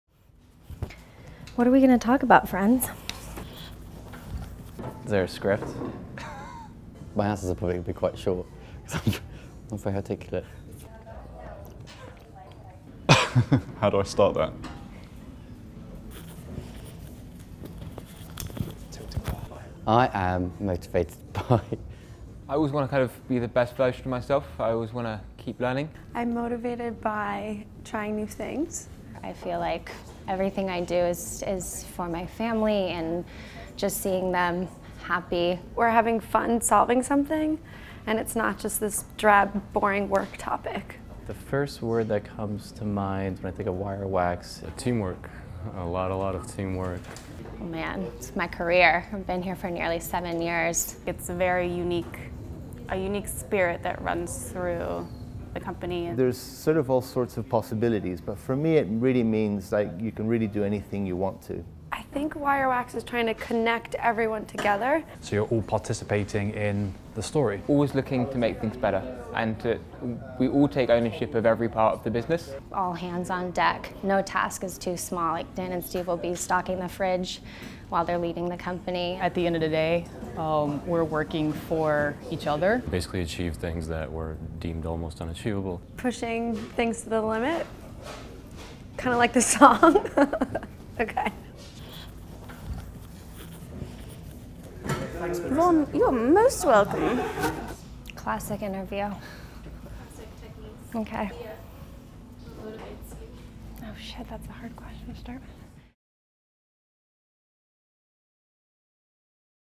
Meet WIREWAX
The people of WIREWAX are what gives this company strength and character to continually push the boundaries of everything we do.

We're a team of creators, disruptors thinkers, and doers working side by side to bring an evolution to the entire world of video.